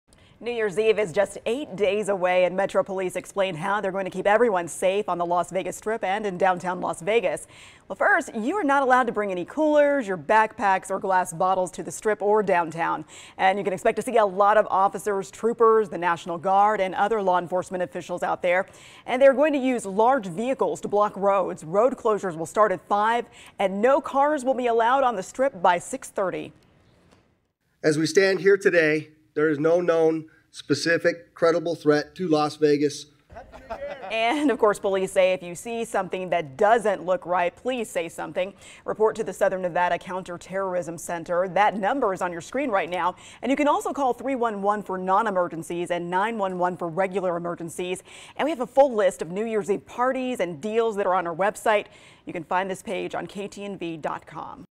Las Vegas police announce security plan for New Year's Eve
New Year's Eve is eight days away, and Las Vegas police discussed how they plan to keep everyone safe during the celebrations.